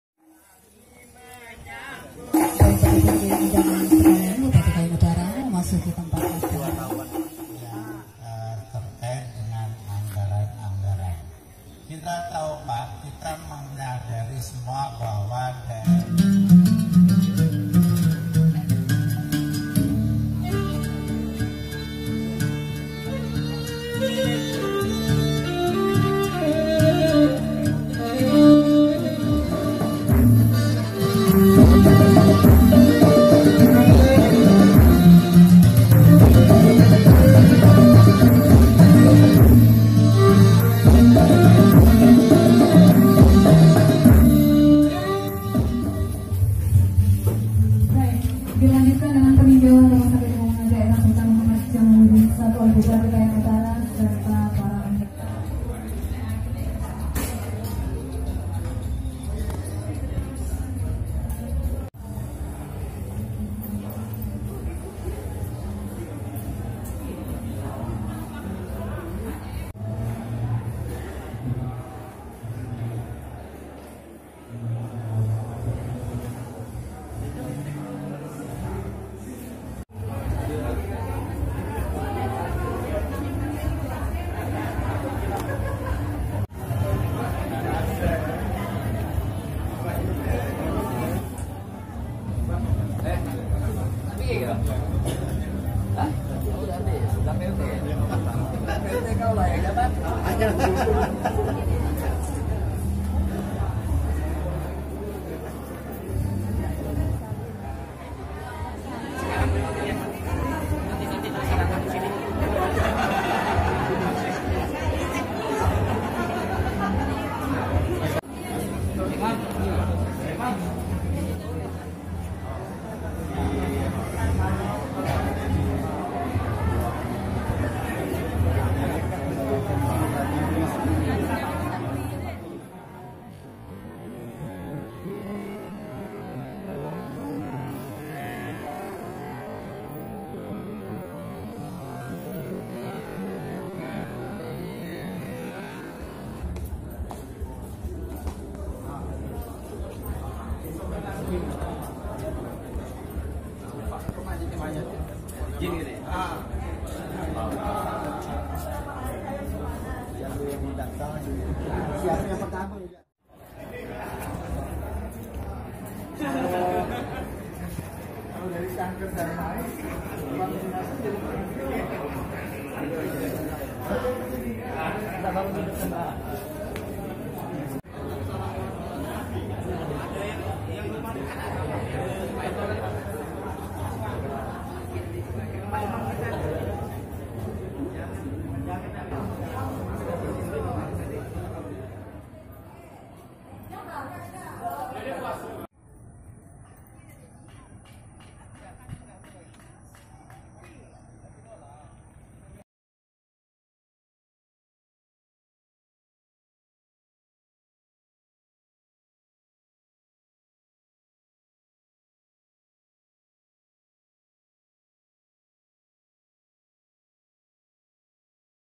PERESMIAN RSUD SULTAN MUHAMMAD JAMALUDIN I
Peresmian
RSUD SULTAN MUHAMMAD JAMALUDIN I
Oleh
Bupati Kab. Kayong Utara